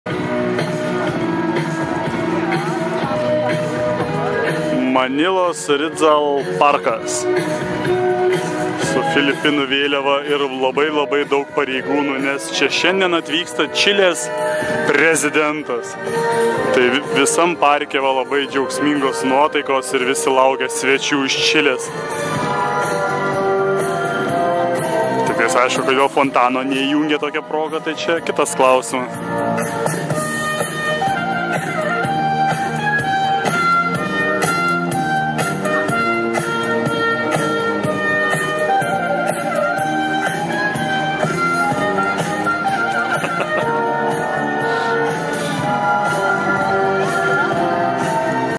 2015-11-16 Evening walk in Rizal park, Manila, Philippines
Rizal parke buvo daug žmonių (visi vietiniai), grojo tyli muzika, viskas buvo labai kruopščiai sutvarkyta. O geriausiai atrodė ta filipinietiškų vėliavų alėja, prie kurios daugiausiai nuotraukų ir pasidariau. Tos vėliavos ratu juosė mažą parko dalies fragmentą, kurio pabaigoje stovėjo kažkoks keistas paminklas su garbės sargybiniais iš abiejų pusių.
Piktinausi, kad neveikia spalvotieji fontanai bei stebėjau kaip su manim koketuoja visos filipinietės iš eilės - tiek jaunos, tiek, deja, ir senos (video užfiksuotos pastarosios).